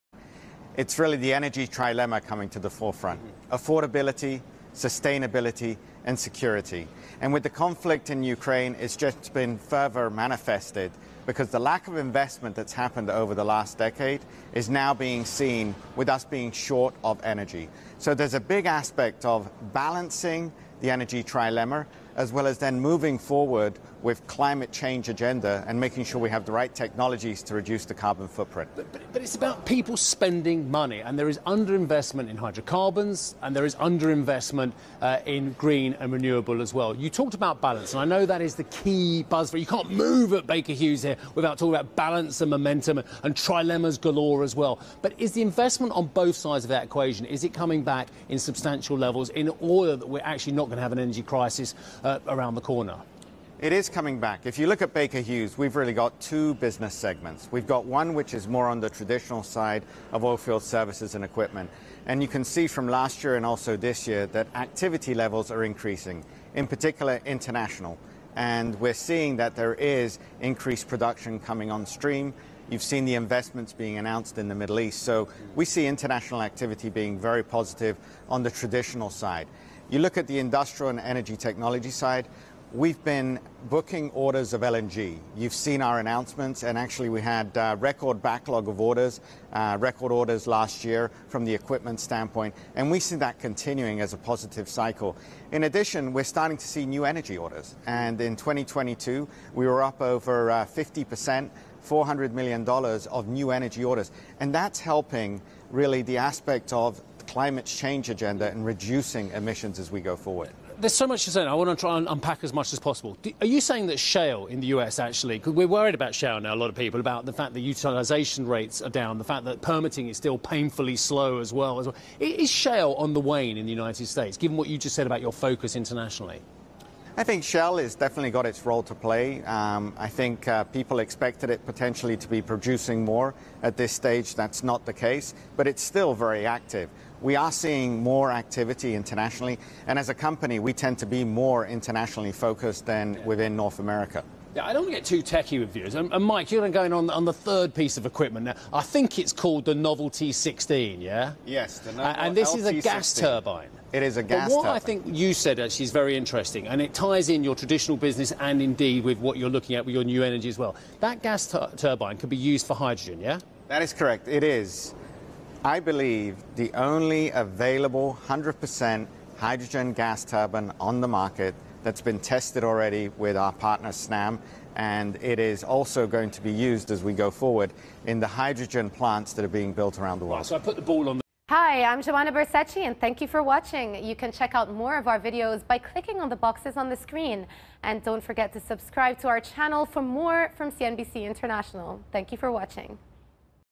Oil and gas industries are back, says Baker Hughes CEO
Lorenzo Simonelli, CEO of Baker Hughes, joins CNBC for an exclusive interview at this year's Baker Hughes conference and what he foresees for the gas and oil sector in 2023.